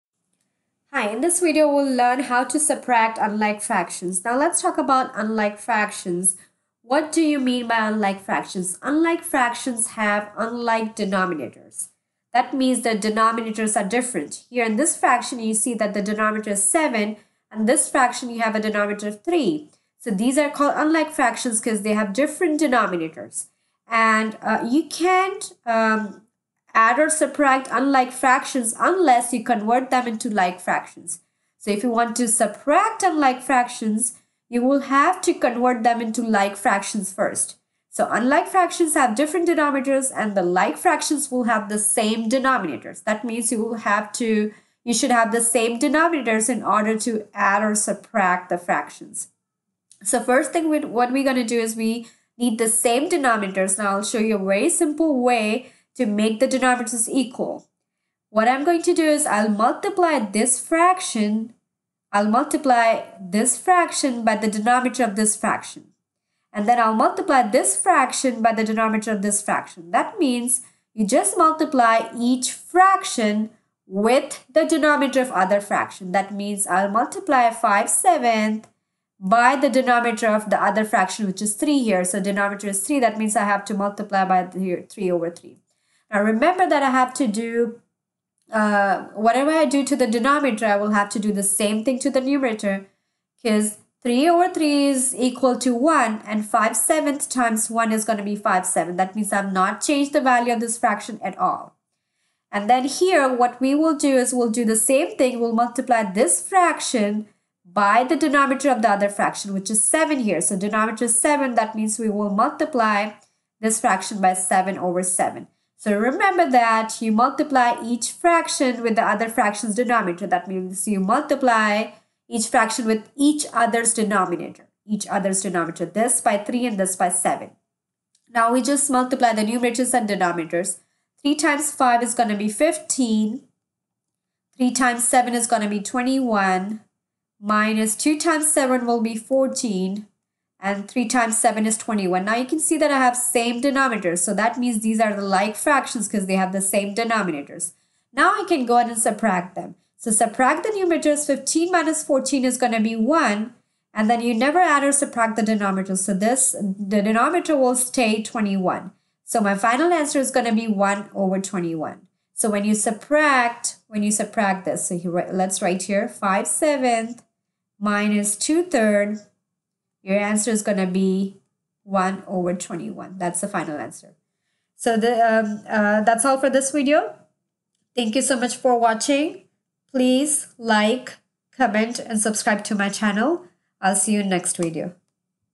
How to subtract unlike fractions | subtracting fractions with different denominators
This video teaches how to subtract unlike fractions or subtracting fractions with different denominators.